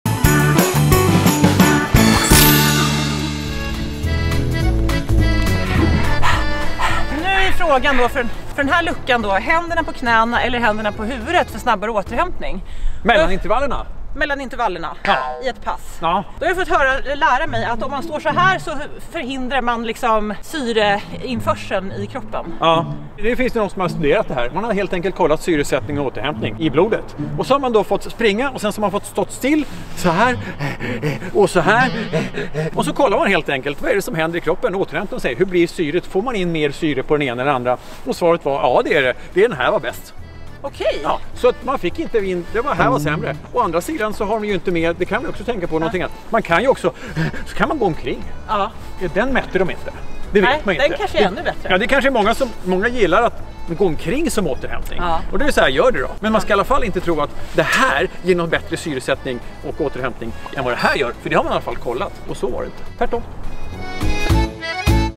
LÖPARJULKALENDERN #8: Rätt sätt att pusta ut!
HO HO HO!! Världens bästa julkalender med löpteknikexperten Fredrik Zillén och mig är här! Varje dag, hela vägen fram till julafton, ger vi dig smarta och roliga tips som tar din löpning till nästa nivå! Hoppas du vill hänga med oss, för nu kööööör vi!! Tipsen passar löpare oavsett nivå, nybörjare till elit. Löparkramar från oss!